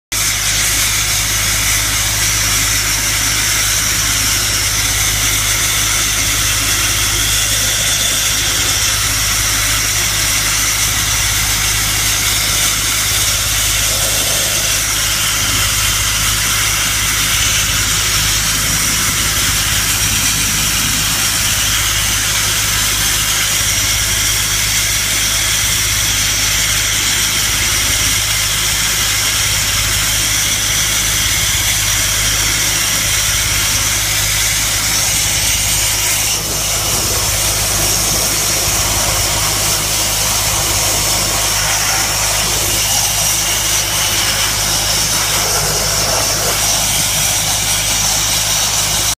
April 21, 2017 - Sandblasting a 60's Camaro body, frame and undercarriage.
Rob Sandblasting the body, frame, and undercarriage, of a late 60's Chevy Camaro, in Churchville MD.